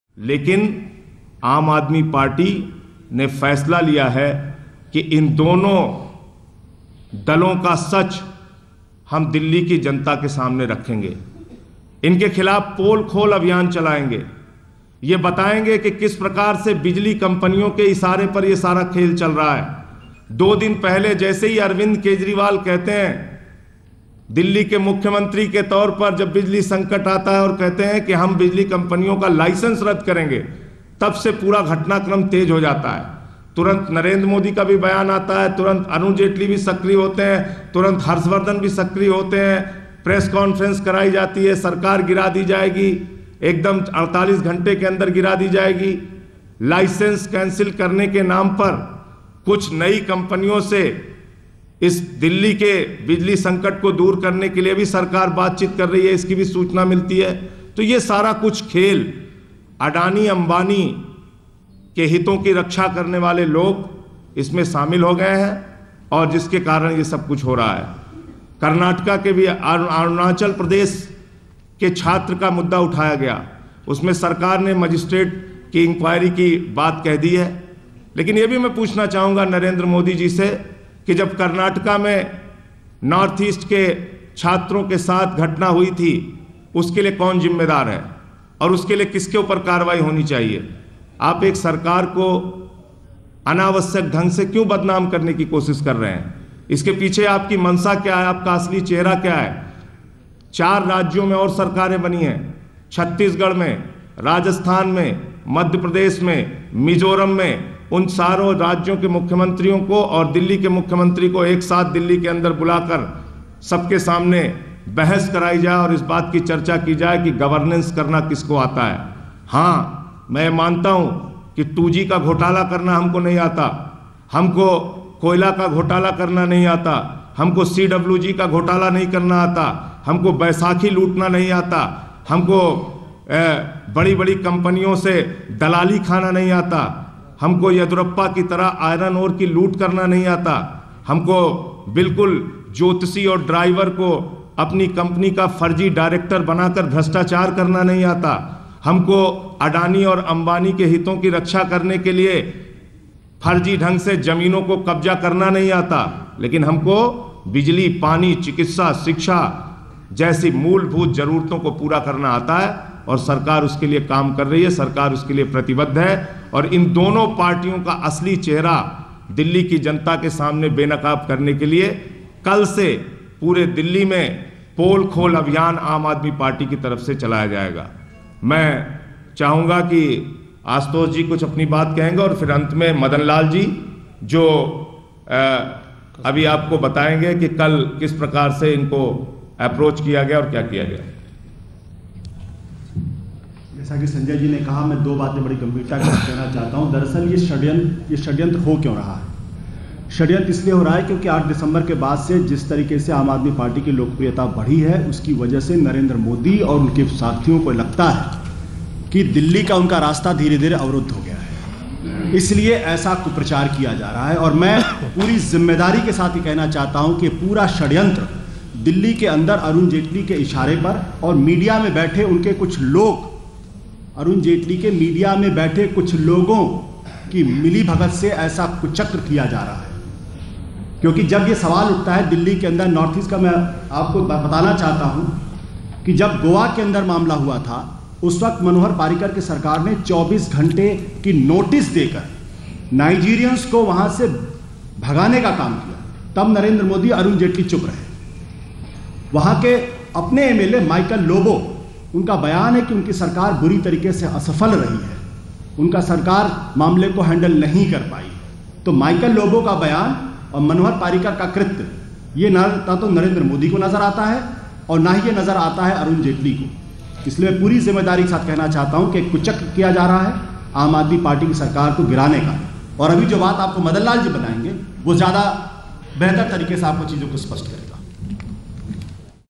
AAP announces 'Poll Khol' campaign in Delhi
Addressing the media, AAP leader Sanjay Singh announced a 'Poll Khol' campaign in Delhi to bring out the truth of BJP and Congress. He accused the BJP and Congress of being hand-in-glove with power distribution company due to which Delhi is facing power crisis. AAP leader, Ashutosh believes that the reason behind conspiracies is the rising  popularity of AAP which is a big threat to Narendra Modi. Taking responsibility of his statement, Ashutosh claimed that all his conspiracies are the handiwork of BJP leader Arun Jaitley in connivance with few media personnel.